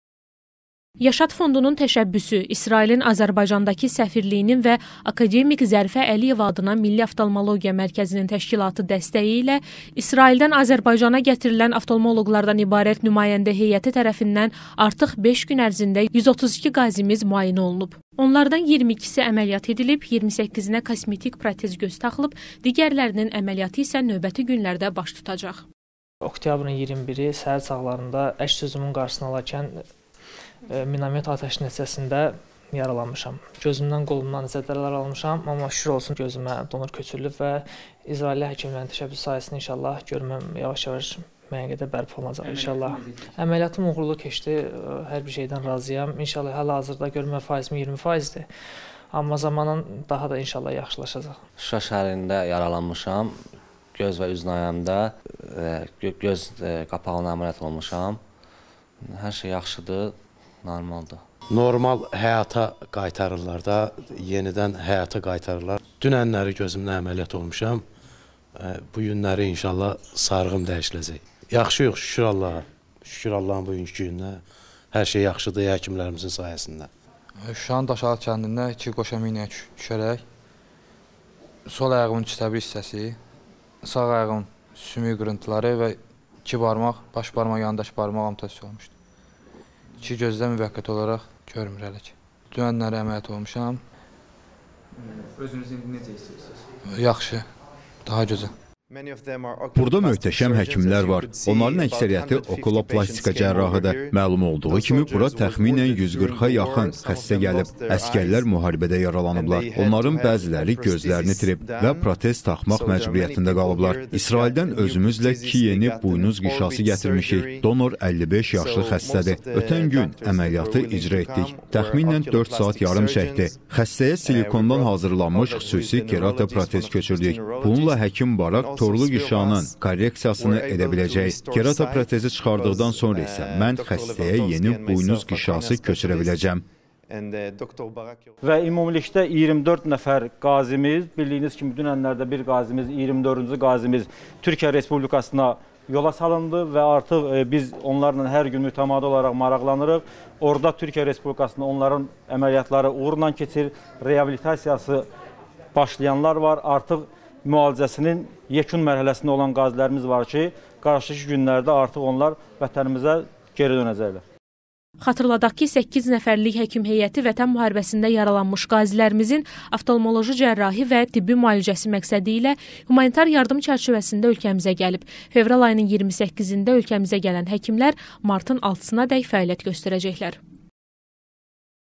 Qazilərimizin müayinələri davam edir
5 gün ərzində israilli həkimlər tərəfindən 132 qazimiz müayinə olunub